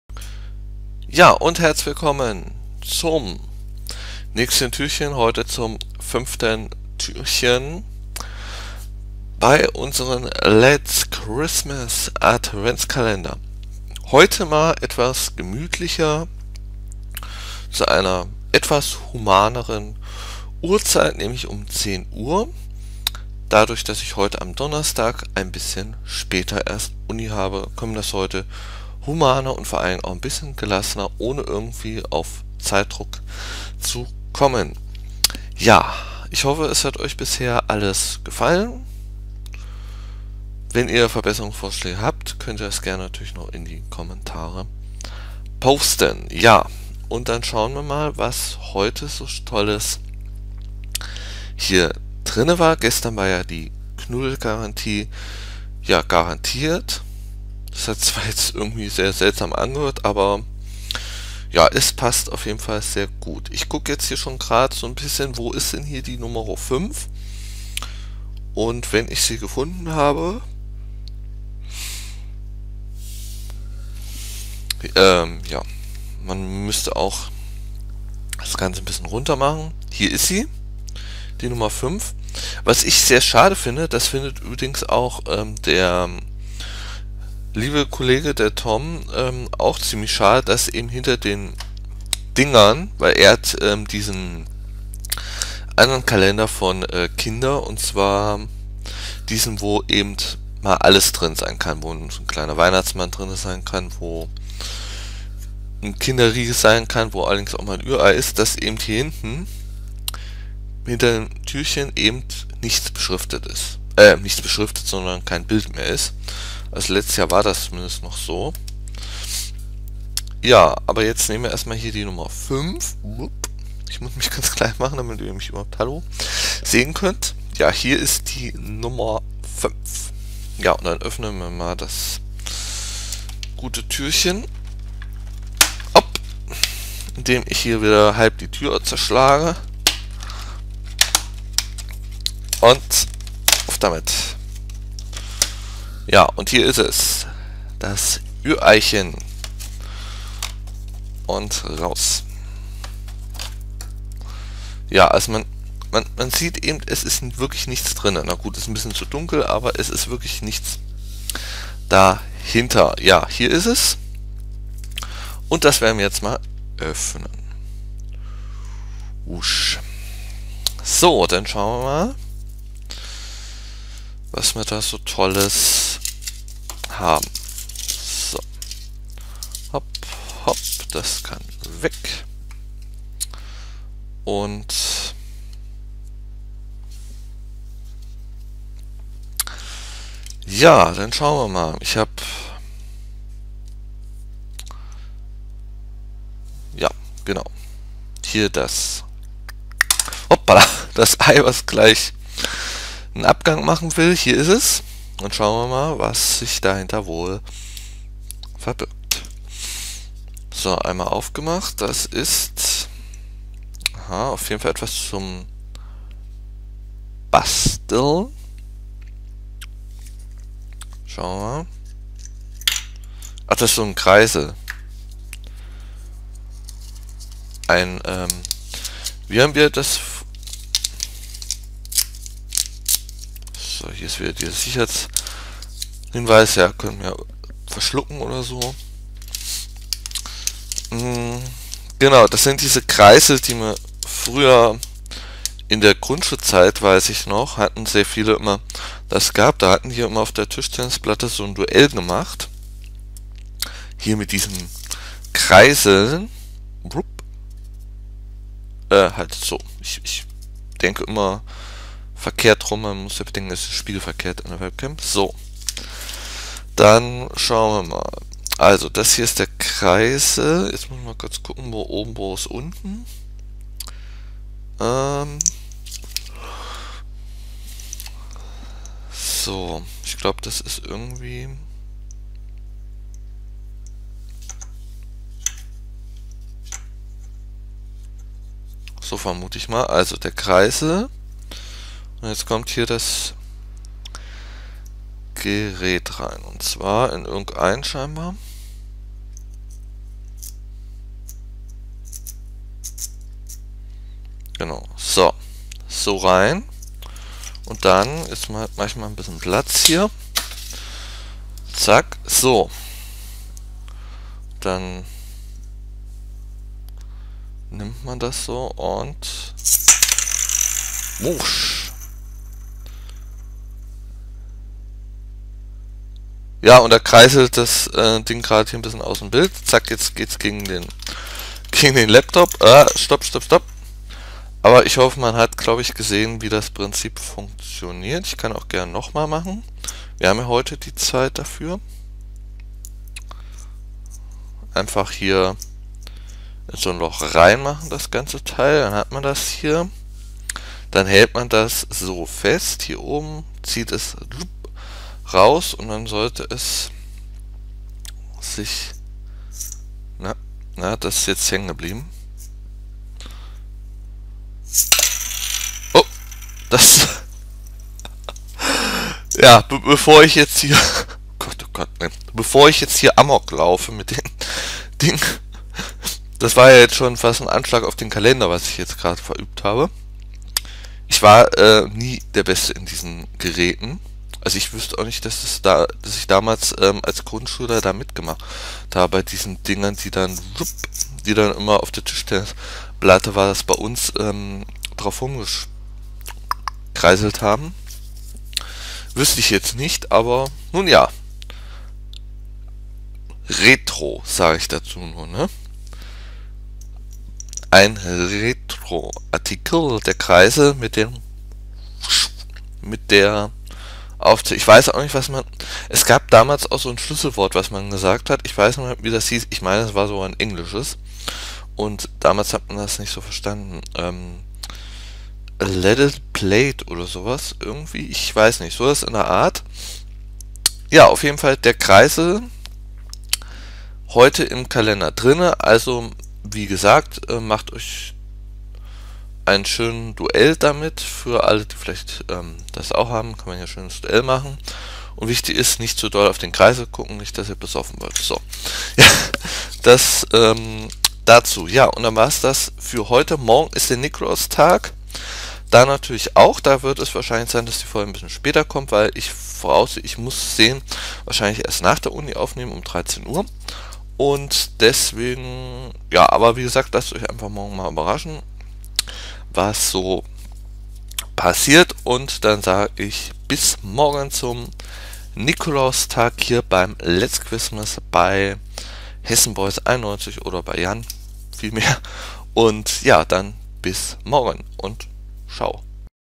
Let's Christmas [German/HD] - 5. Dezember 2013
|Let's Christmas|
Jeden Tag wird ein Türchen im Üeier-Adventskalender geöffnet :)